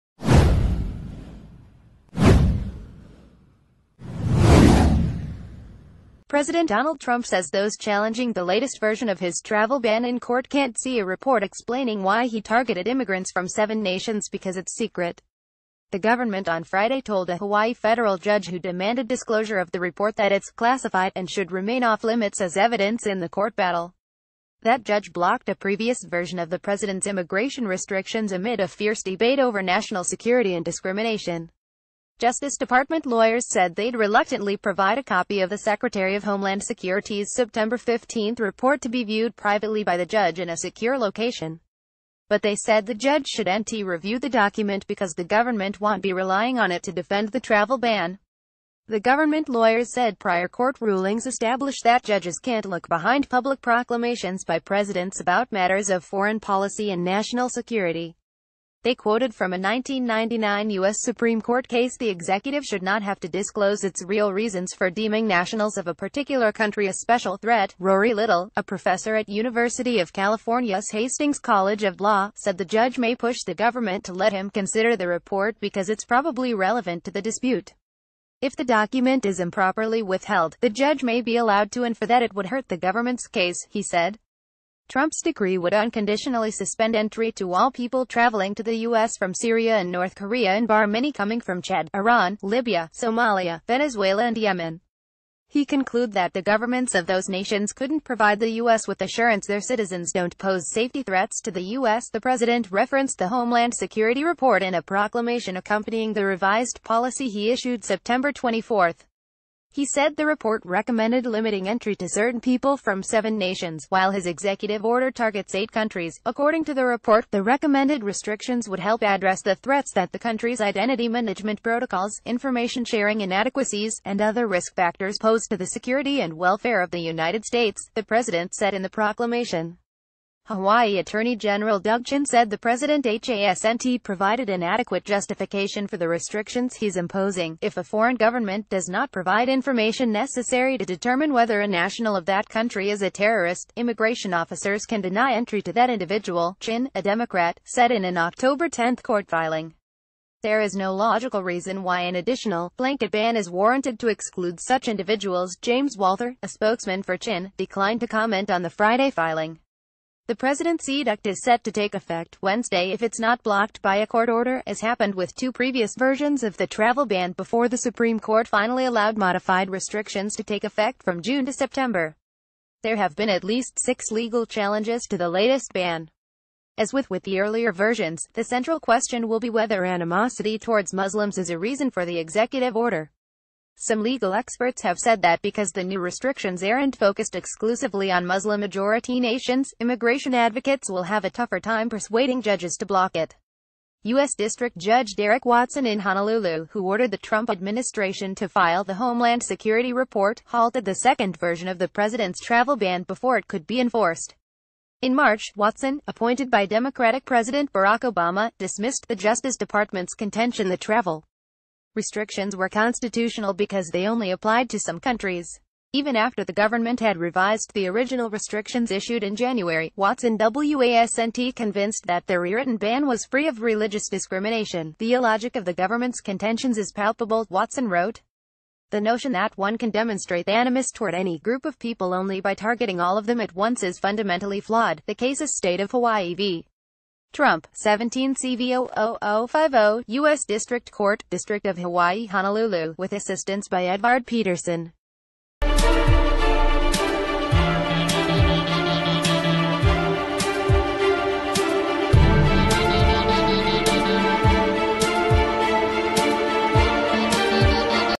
Breaking News | Trump balks at showing classified report backing travel ban
Trump balks at showing classified report backing travel ban

President Donald Trump says those challenging the latest version of his travel ban in court can’t se...